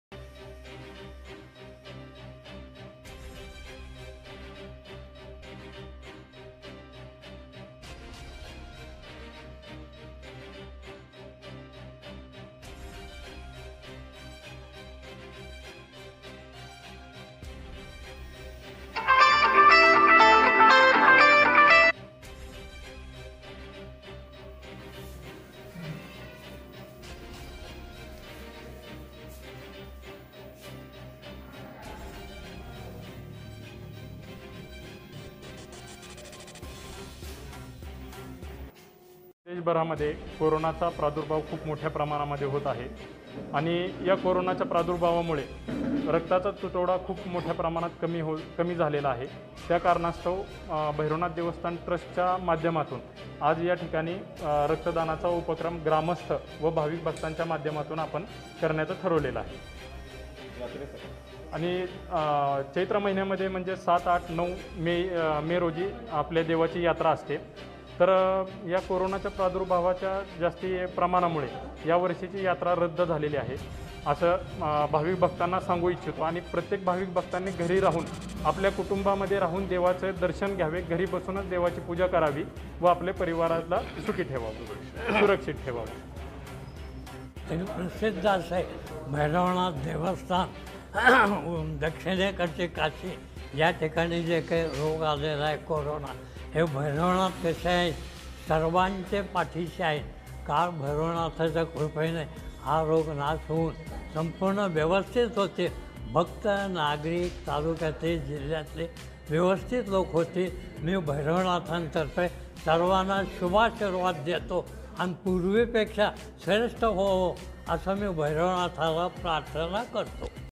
सोनारीच्या काळभैरवनाथची यात्रा रद्द, यात्रेवर होणारा खर्च टाळून रक्तदान शिबिराचे आयोजन
सोनारीच्या काळभैरवनाथची यात्रा रद्द, राज्यात रक्ताचा तुटवडा असल्याने यात्रेवर होणारा खर्च टाळून रक्तदान शिबिराचे आयोजन